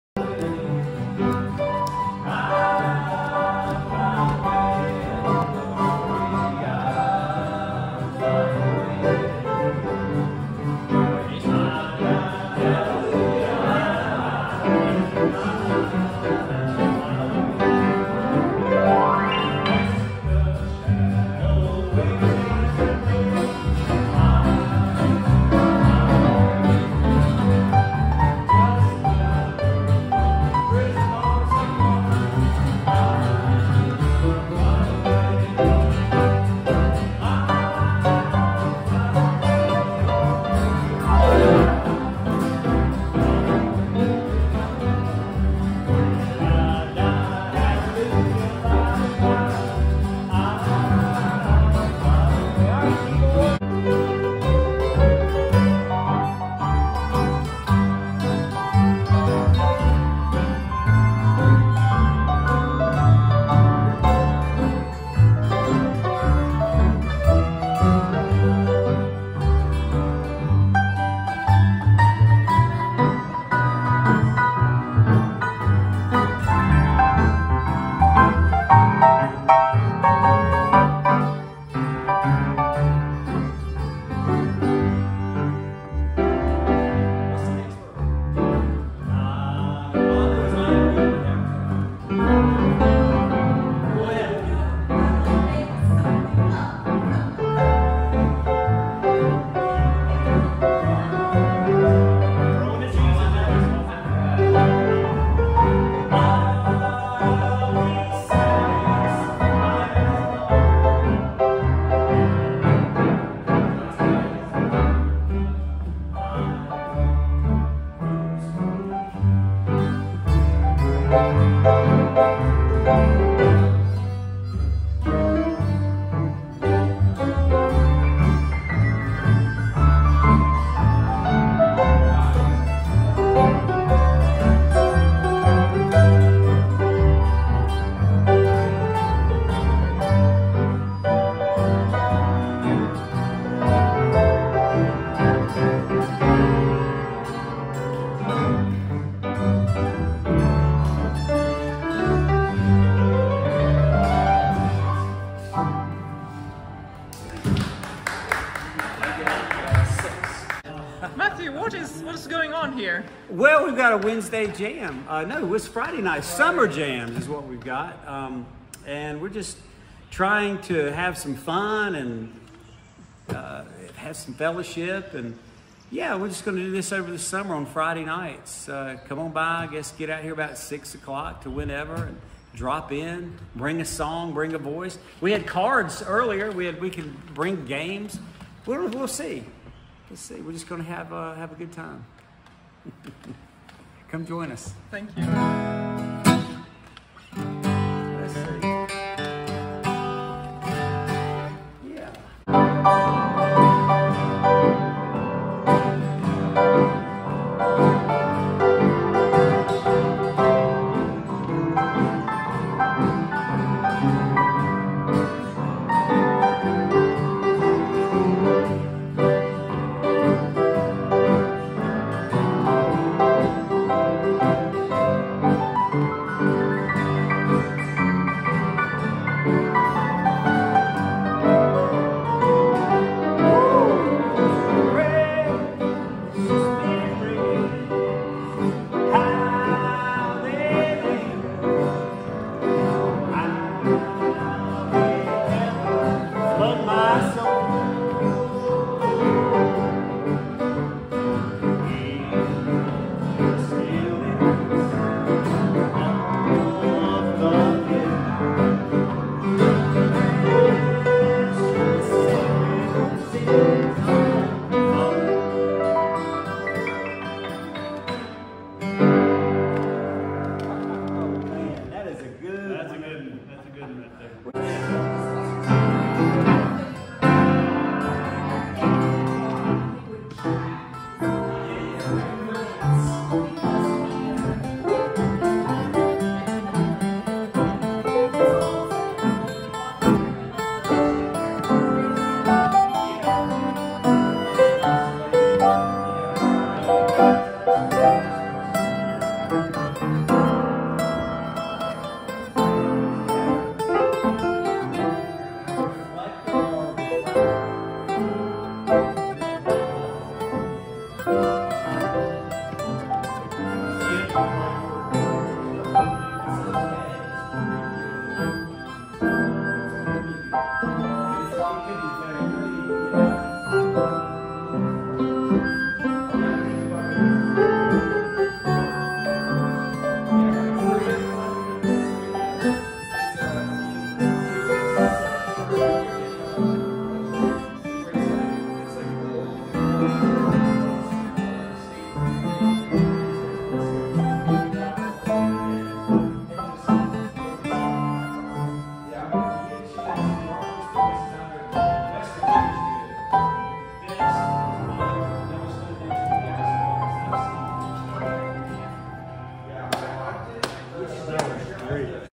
Summer Jams at Emmanuel, May 25, 2022
A little bluegrass, a little cribbage, a little fellowship on Friday nights.